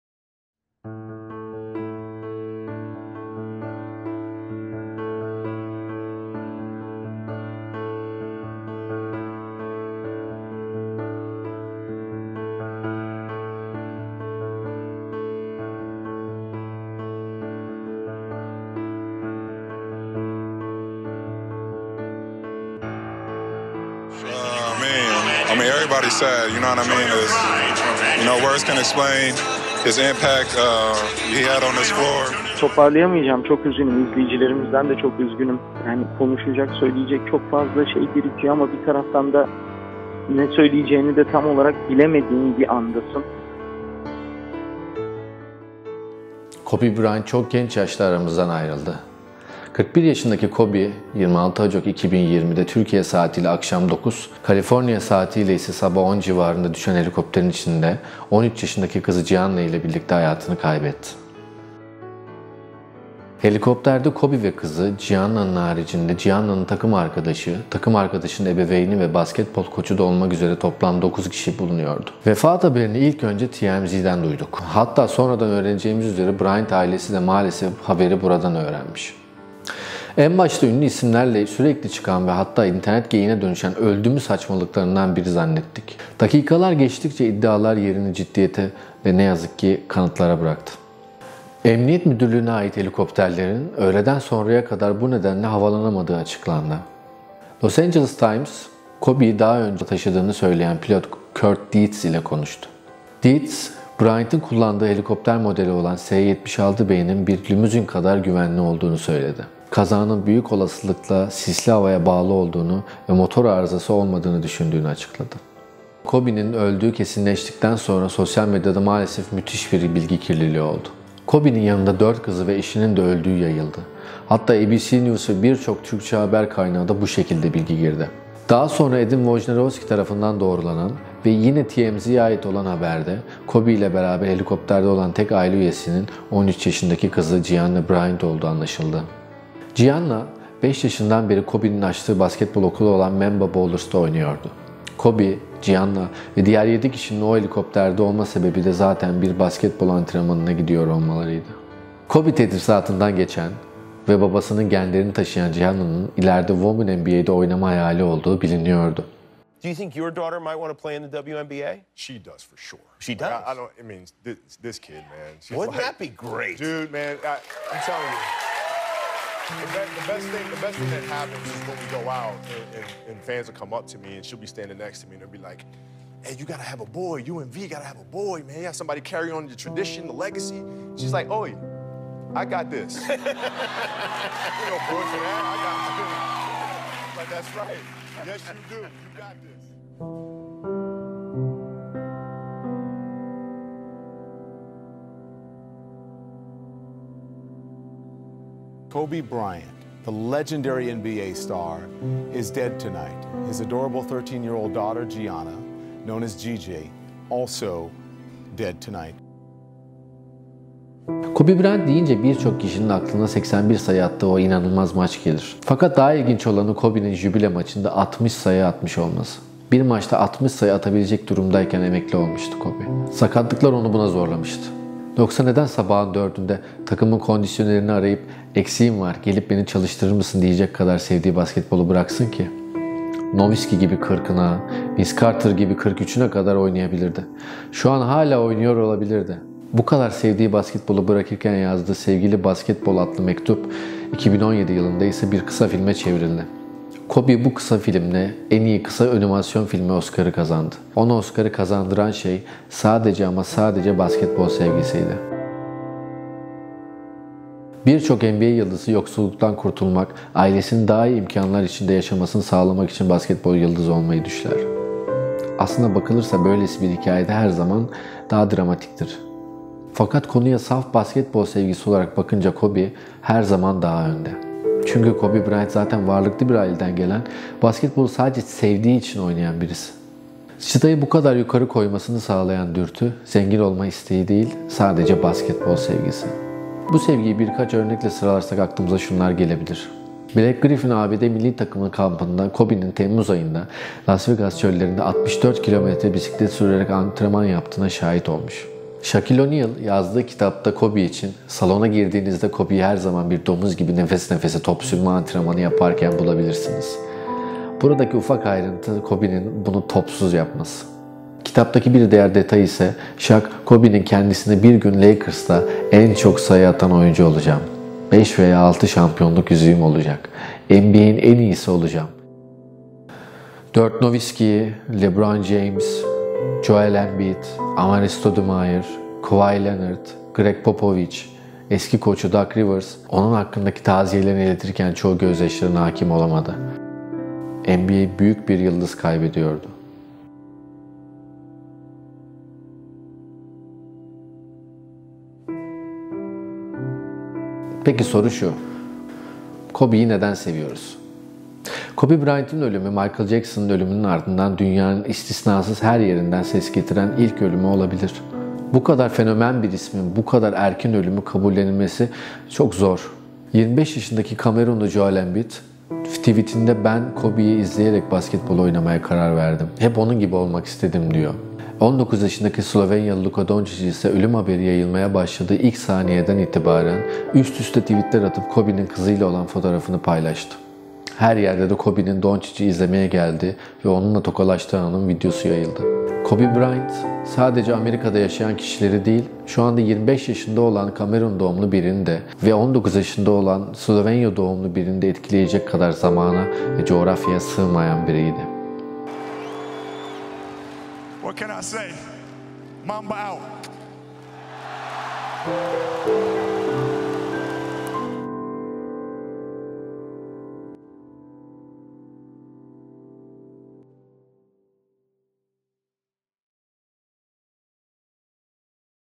Özel Dosya | Kobe Bryant'ın Ölümü ve Arkasında Bıraktıkları
90'ların sonu, 2000'lerin başı, yani milenyum, nedense birçok insan için farklı anlamlar taşıyor. Amerika'da da Türkiye'de de bu böyle.
İşte Kobe Bryant, bu dönemlerde basketbolu tüm dünyadaki ve çocuklara sevdiren isimdi.
25 yaşındaki Kamerunlu Joel Embiid, tweet'inde "Ben Kobe'yi izleyerek basketbol oynamaya başladım. Kobe gibi olmak isterdim" diyor.
19 yaşındaki Slovenyalı Luka Doncic de ölüm haberi yayılmaya başladığı ilk saniyeden itibaren üst üste tweet'ler atıp Kobe'nin kızıyla olan fotoğrafını paylaştı. Her yerde de Kobe'nin Doncic'i izlemeye geldiği ve onunla tokalaştığı anın videosu yayıldı.
Kobe Bryant, sadece Amerika'da yaşayan kişileri değil, şu anda 25 yaşında olan Kamerun doğumlu birini de, şu anda 19 yaşında olan Slovenya doğum birini de etkileyecek kadar zamana ve coğrafyaya sığmayan biriydi.